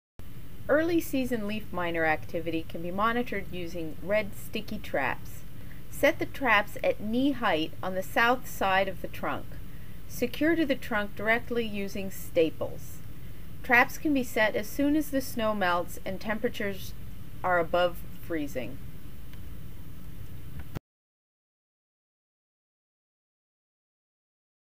Early Season Leafminer Traps
Morgan shows how to put up a red sticky trap to help monitor early season leafminers in the orchard.

Citation:  Berkett, L.P.  "Early Season Leafminer Traps".  20 Apr 2011.  Online video clip.  UVM Fruit Program YouTube Channel.